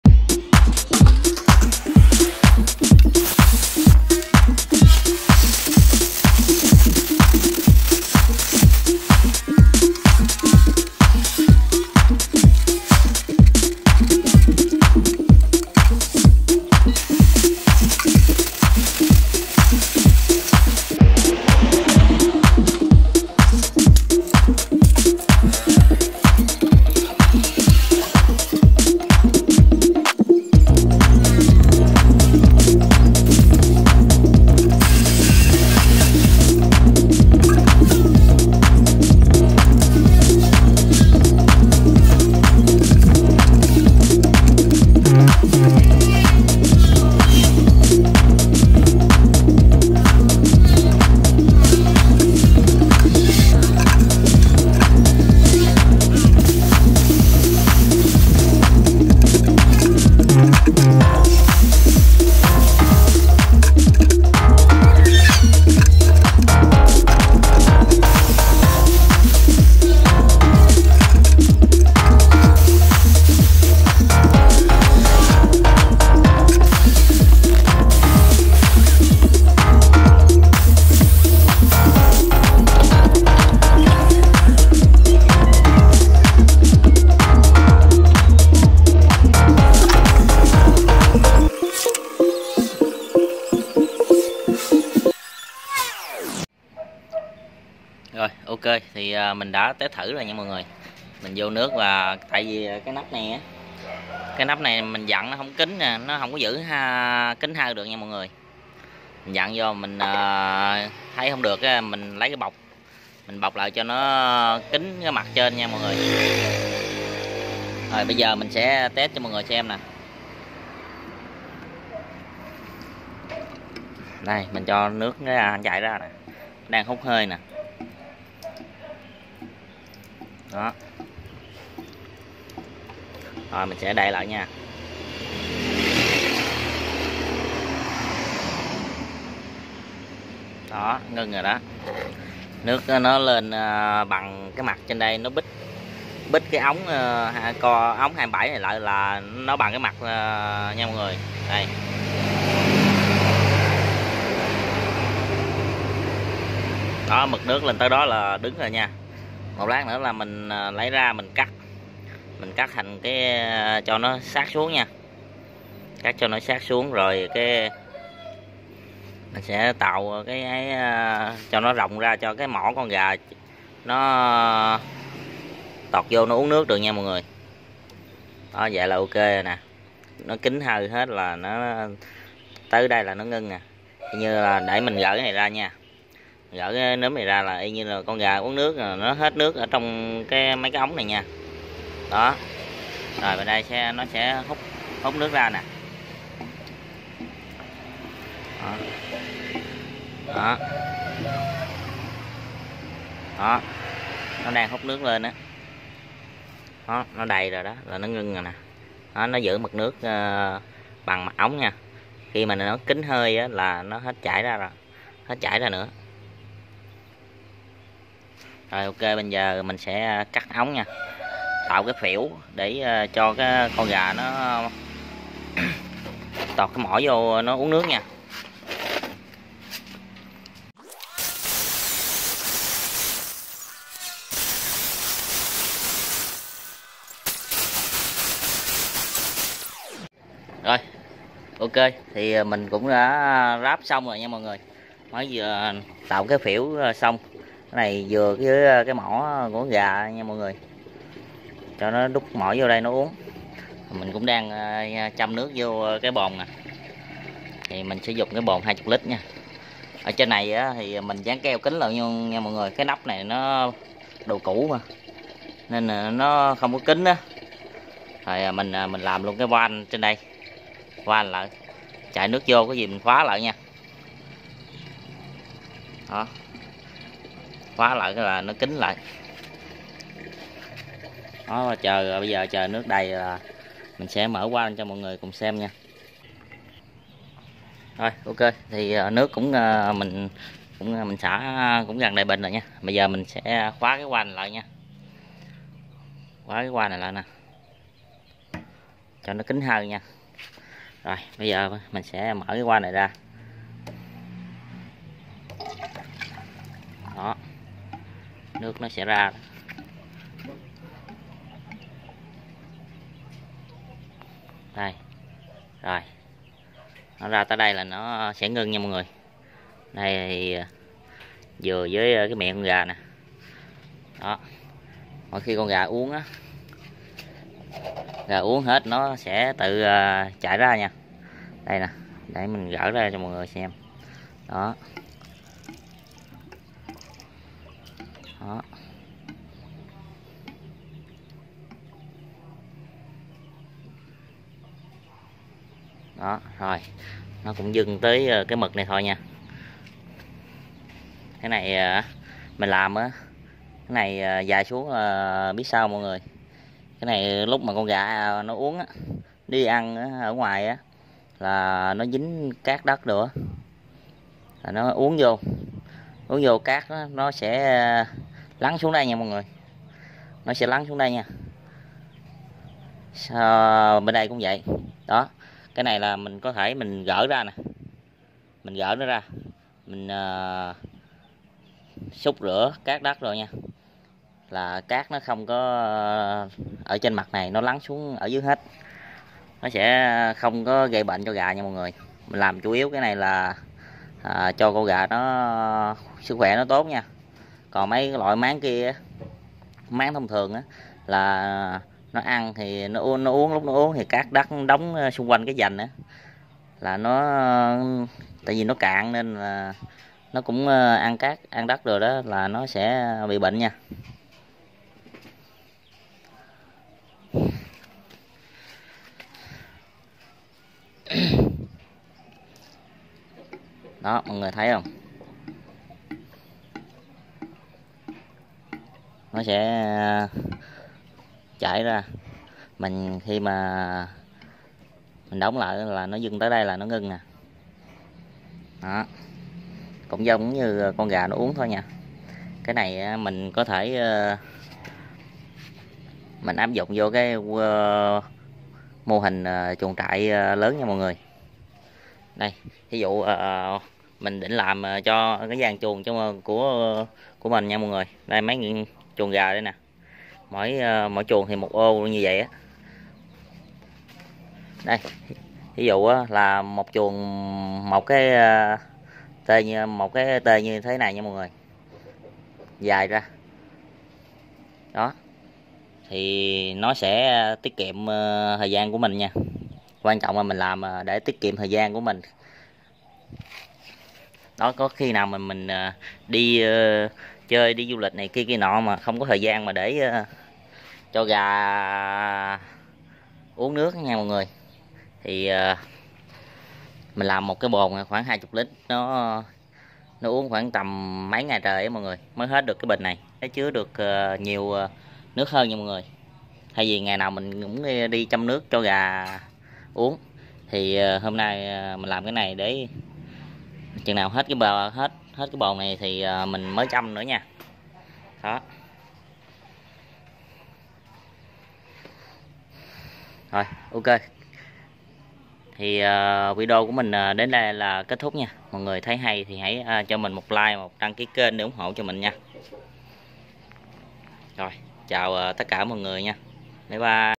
Làm máng nước tự động giảm mầm bệnh cho gà/Make automatic water troughs for chickens
Chia sẻ cách làm máng nước tự động cho gà giảm tối thiểu mần bệnh cho gà